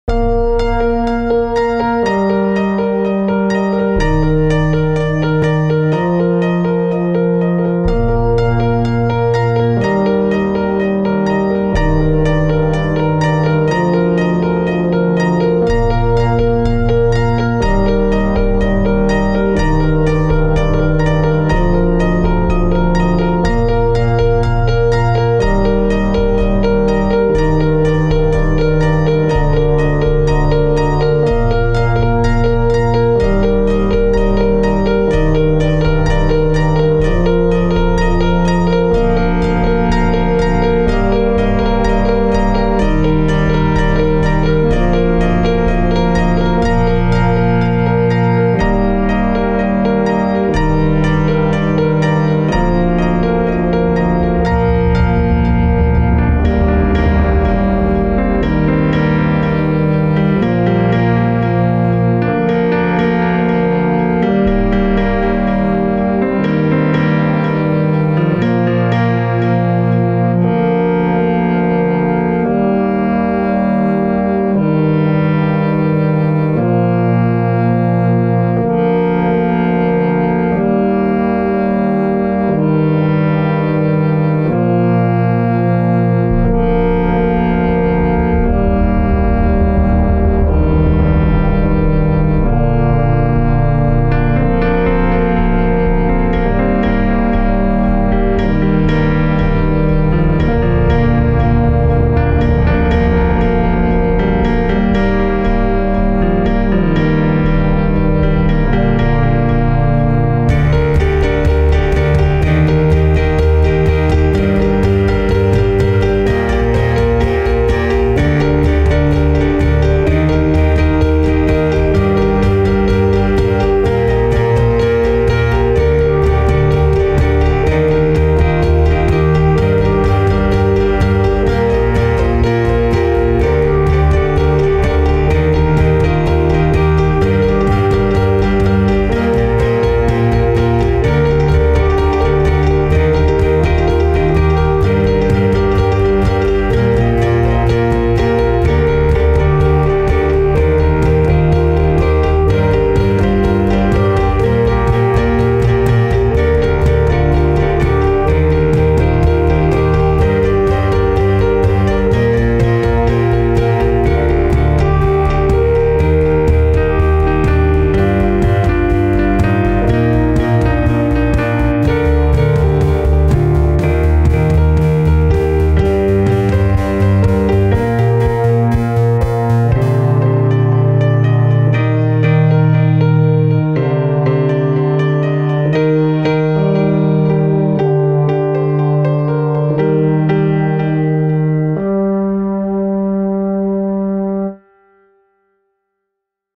A day to remember
An abstract animation of my own with a song i made on Reason quite a while ago.